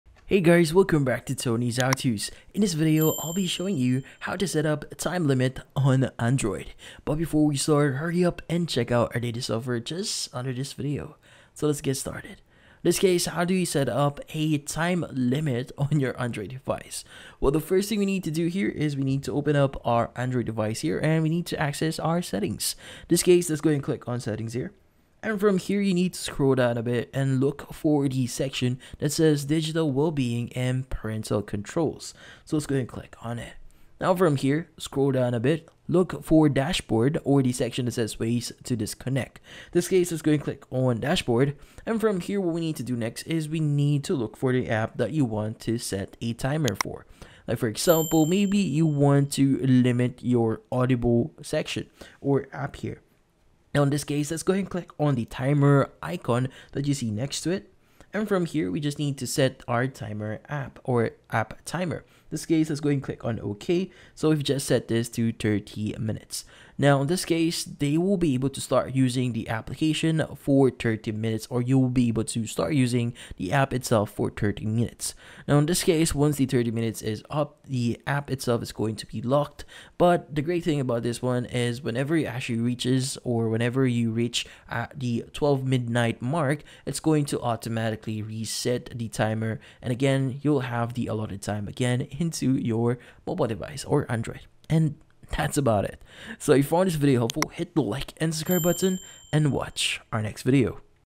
How to Set App Time Limit on Android (2024)
In this video, I show you How to Set App Time Limit on Android in 2024. The quick and easy tutorial How to Set Time Limits for Apps on Android. Do you want to know step by step how to Place a Time Limit on Apps Android in 2024? Yes? Great!